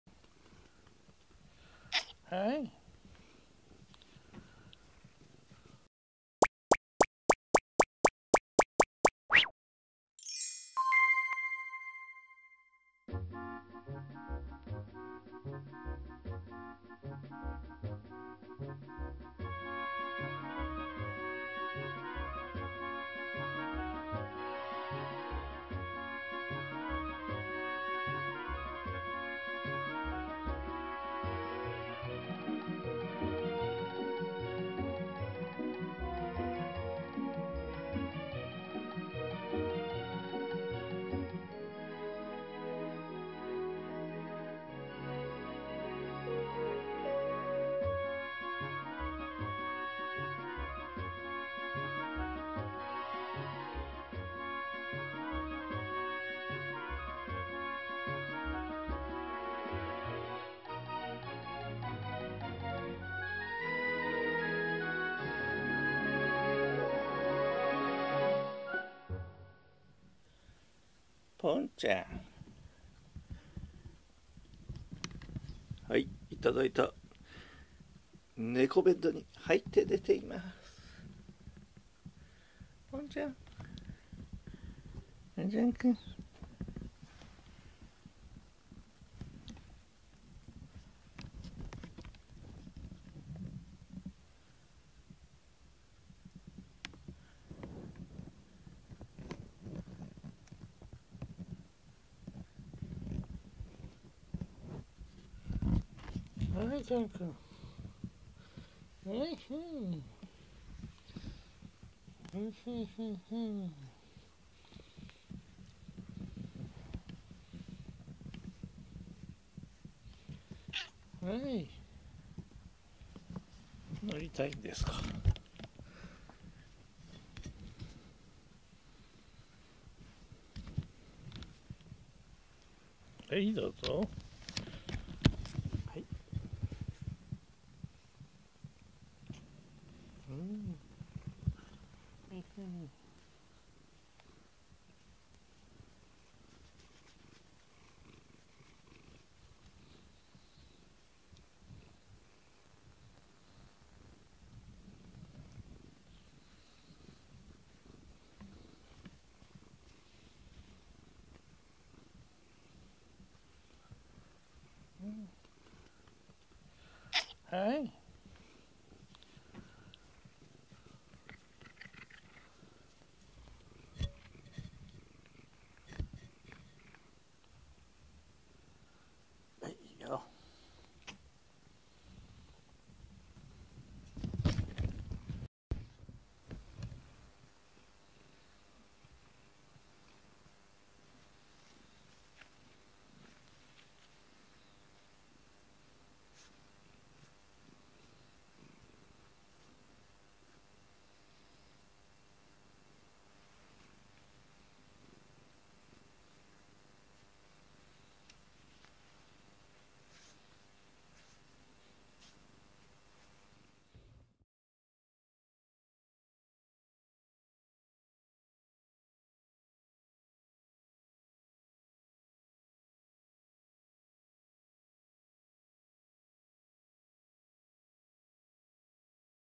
プレゼントにもらった猫ハウスにポンちゃんが入った【Jean & Pont 2729】2022/1/15 保護猫ジャンけんポン
ポンちゃんの好奇心の賜物で新しいキャットハウスを使ってもらえることになりました。
もう必ずと言っていいほど初めて使うのはポンちゃんでジャンくんは100%安全を確認してからで時間がかかります。
せっかく頂いたキャットハウスに入ってくれて嬉しい限りです。
はい、ジャンくん道がついたよ！